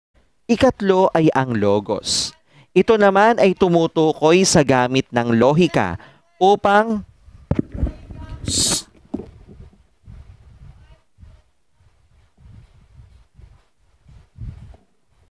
Video Lesson Recording Problem
Relate siguro mga teachers ng new normal dito. Natawa rin ako sa sarili ko nung makita ko reaction ko sa video. Paano naman kasi, nasa peak ako ng recording tapos sumigaw yung nagtitinda ng pinya. Pasensya na, Manong, alam ko po ginagawa mo po ito para maghanapbuhay. OK lang, nagawan naman ng paraan ☺️